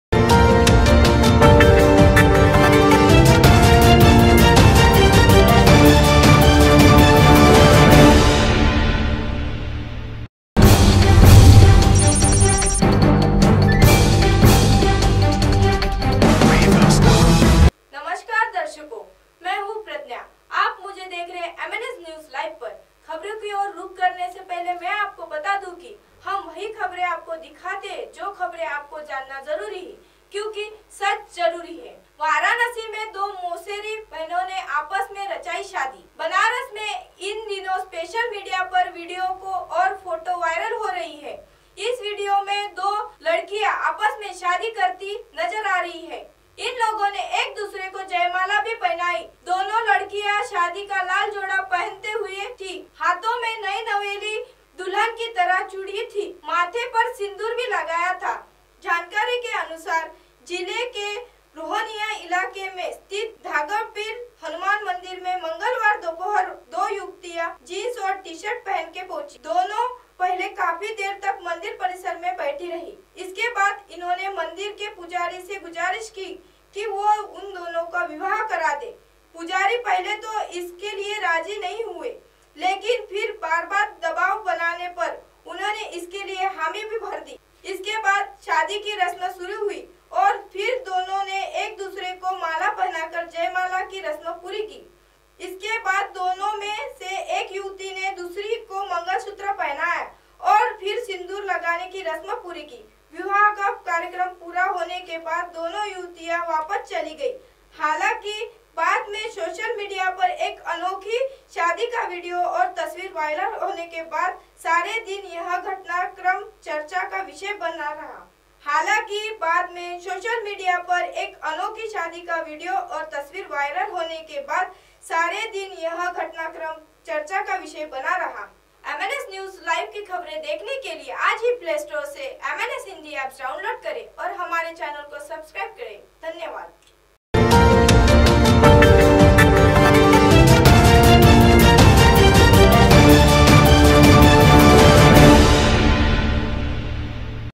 में दो मौसेरी बहनो ने आपस में रचाई शादी
MNSNEWSLIVE वाराणसी में दो मौसेरी बहनो ने आपस में रचाई शादी